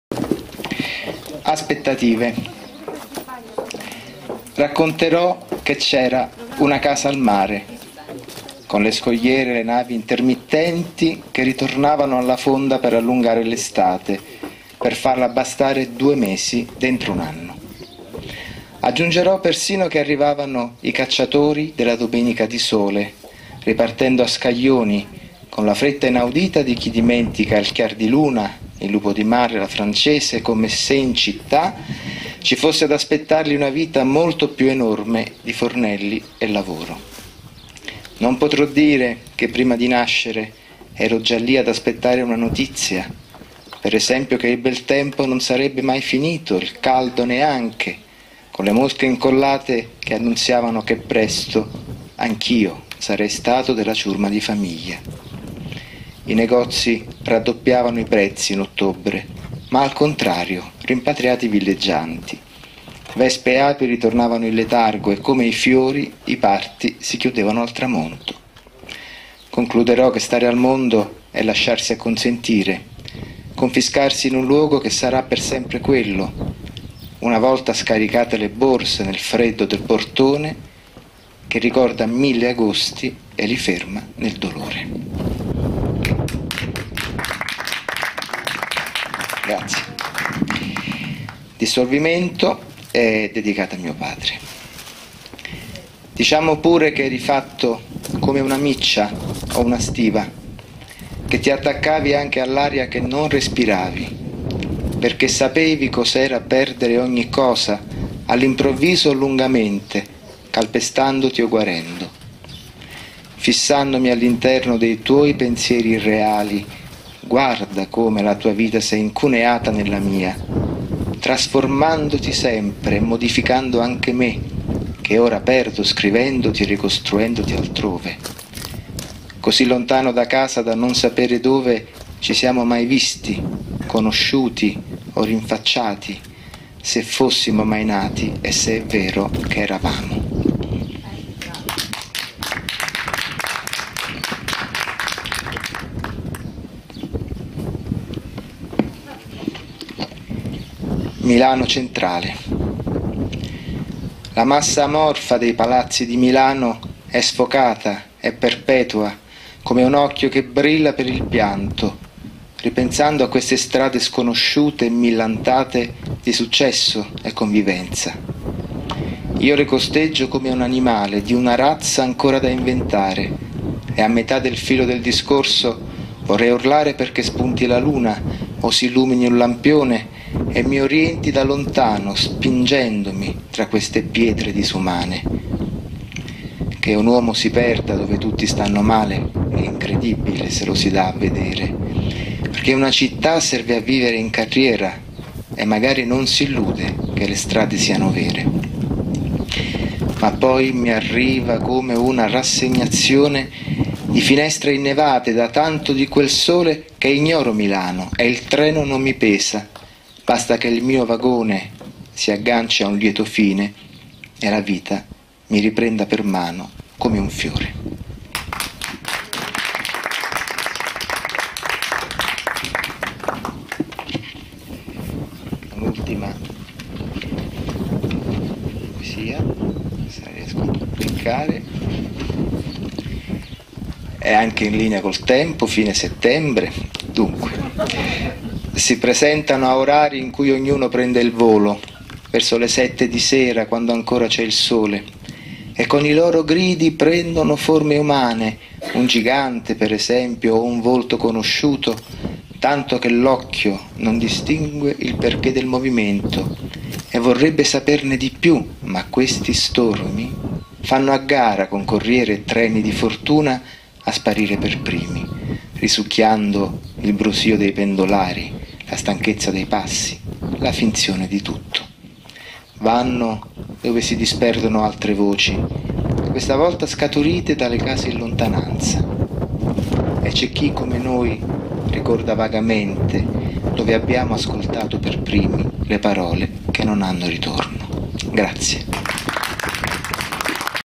Stelvio Di Spigno "Silenzi in forma di poesia 2010"
Stelvio Di Spigno legge sue poesie tratte da "La nudità" (Pequod). 23 maggio 2010, presso 'Torre Nuova', in occasione di "Silenzi in forma di poesia 2010", 3° edizione, rassegna di poesia nazionale contemporanea inserita all'interno del Festival di arti in strada Maggio Sermonetano, (Sermoneta, Latina). Direzione artistica: Bianca Madeccia. Alla terza edizione hanno collaborato gli artisti visivi: Alfonso Marino (poesia visiva e installazioni), Dino Catalano (performance di arte visiva), Angelo Tozzi (installazione), Elio Montagna (installazione).